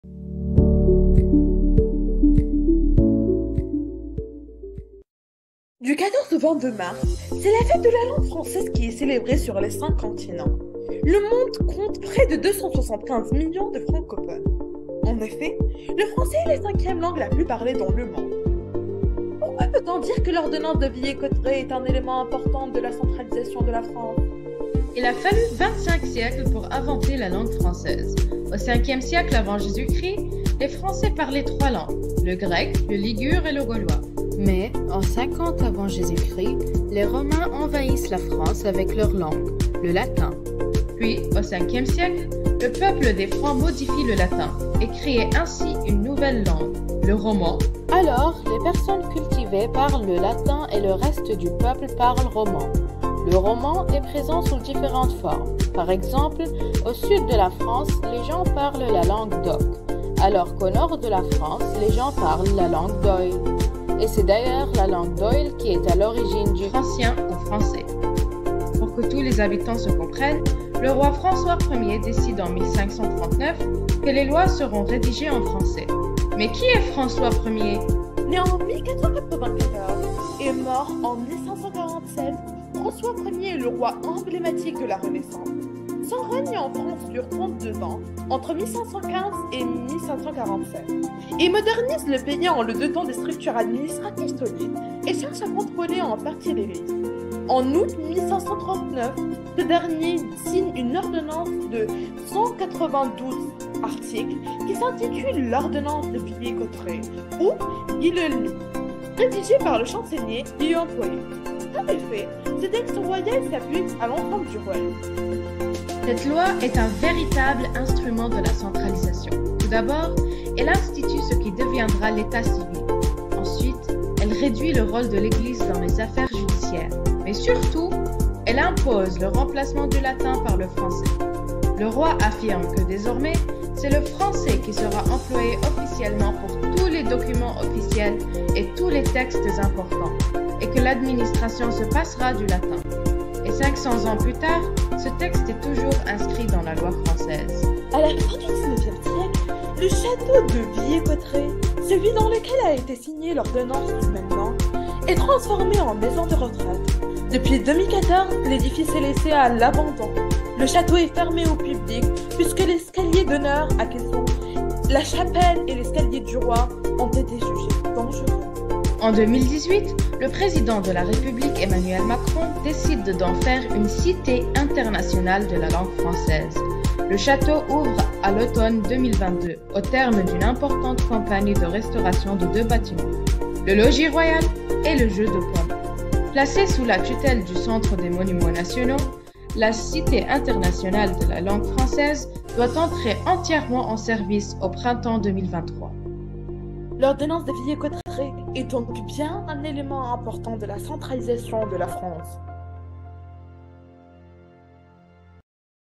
Francophonie
Vidéo réalisée par des élèves de 2nde C , dans le cadre de la francophonie en lien avec le  programme d'Histoire, sur le thème de l'ordonnance de Villers-Cotterêts, qui renforce l'administration royale et le rôle du français.